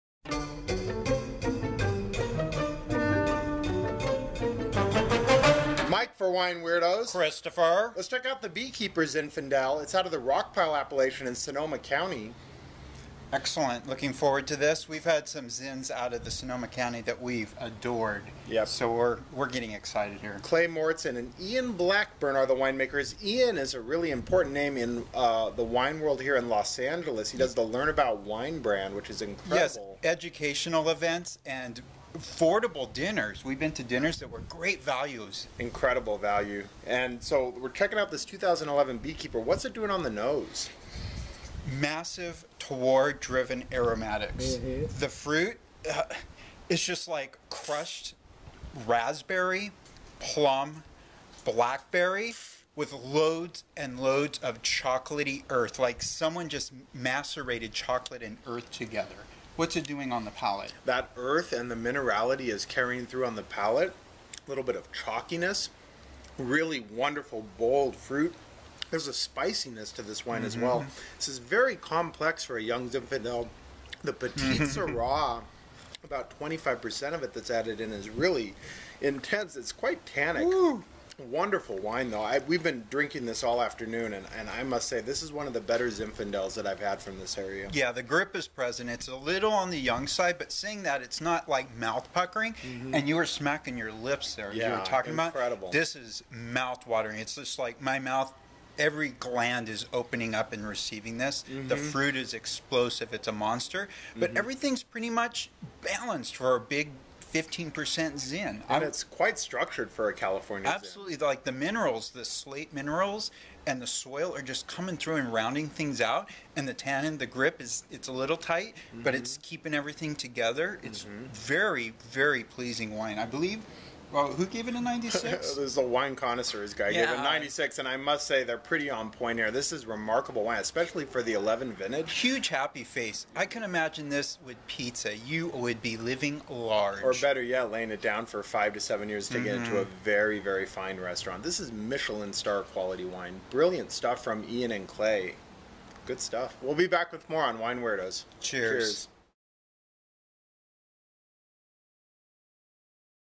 2011 Beekeeper Cellars Zinfandel Madrone Spring High-Quality And Complex Red Wine From Sonoma, CA
Beekeeper Cellars
2011 Zinfandel Madrone Spring
$65.00
California -- Rockpile